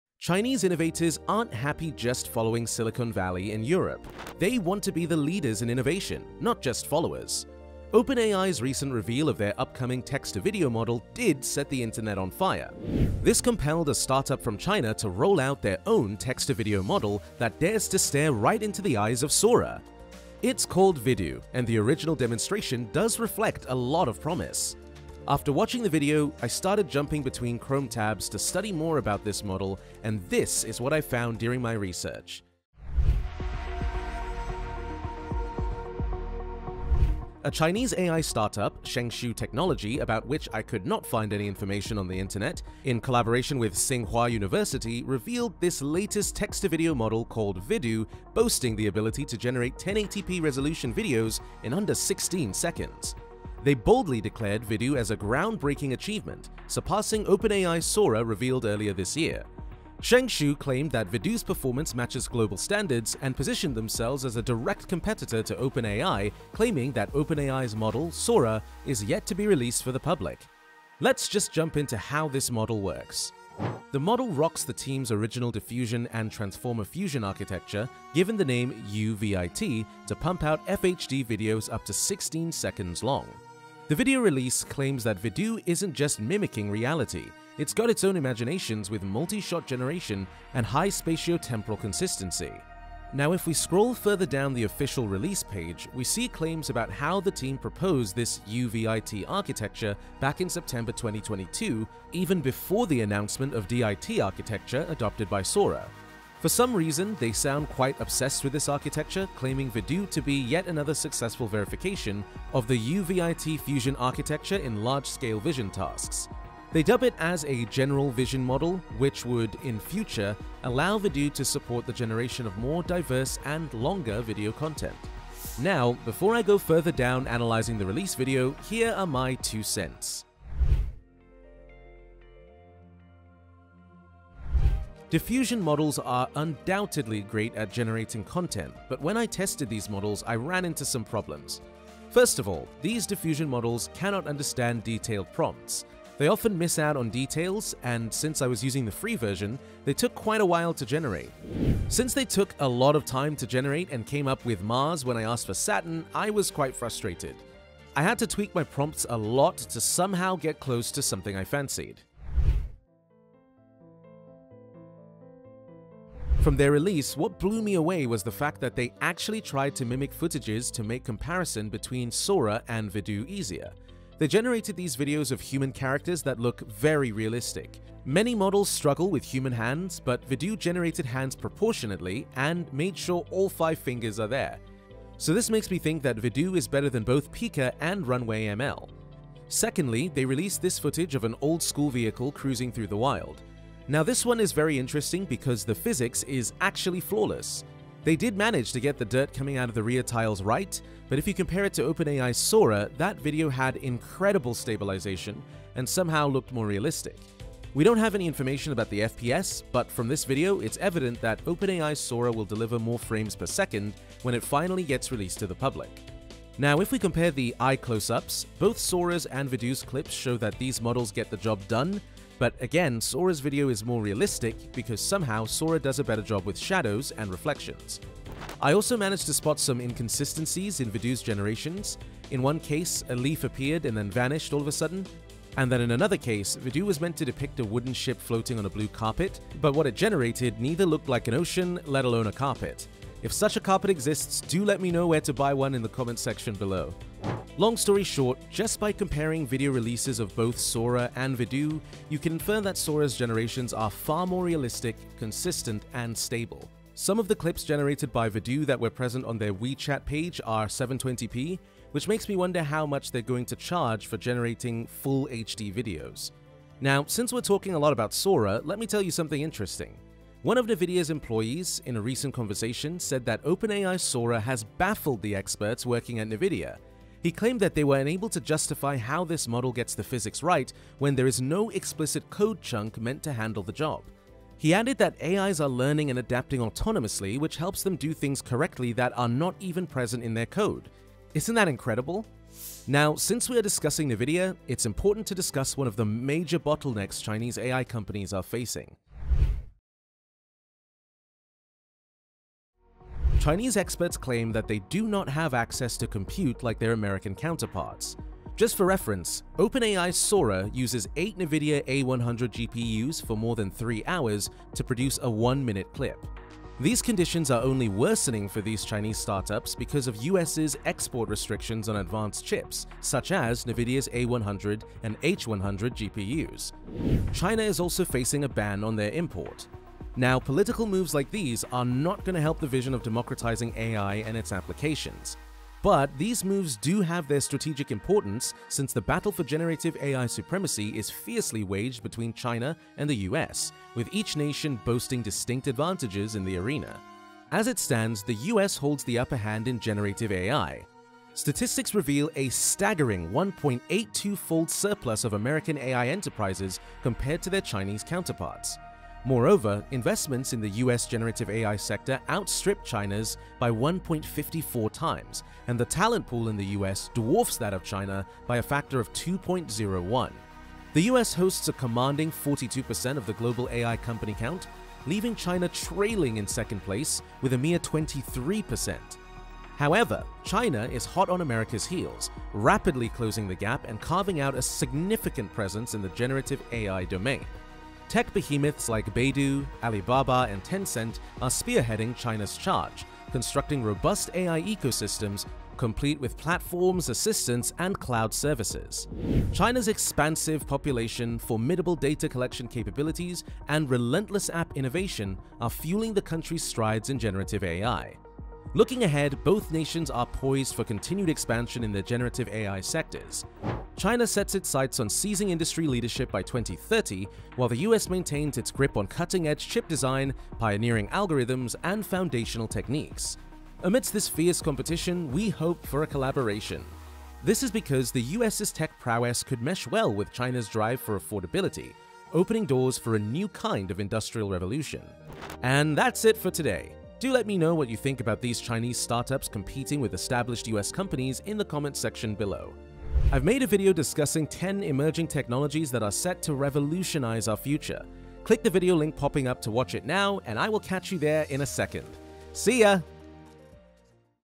The Chinese Competitor to OpenAI's SORA is SHOCKING! (VIDU)
Discover the shocking Chinese competitor to OpenAI's SORA in this video! See how this new technology is changing the game for AI development.